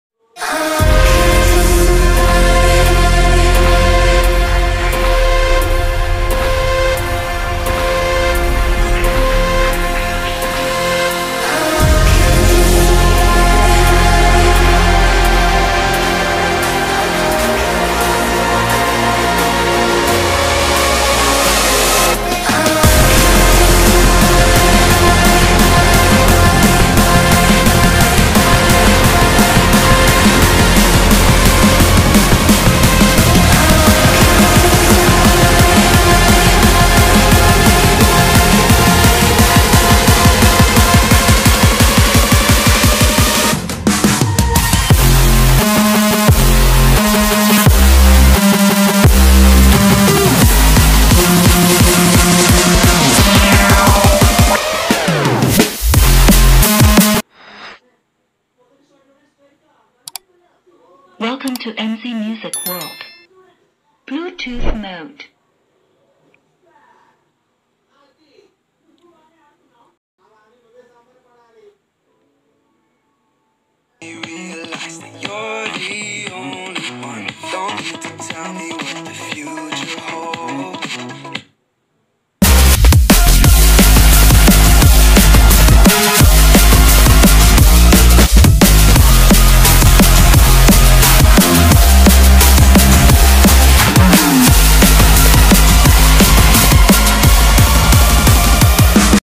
MZ M422SP poRTABLEBLUETOOTH SPEAkER .Batteries :1200mAh 1Lithium -ion 🔋 Battery
MZ M422SP  poRTABLEBLUETOOTH SPEAkER.  Brand.MZ SPeakers

Take a look at this MZ M422SP (PORTABLE BLUETOOTH SPEAKER) Dynamic Thunder Sound with Mobile Stand 10 W Bluetooth Speaker on Flipkart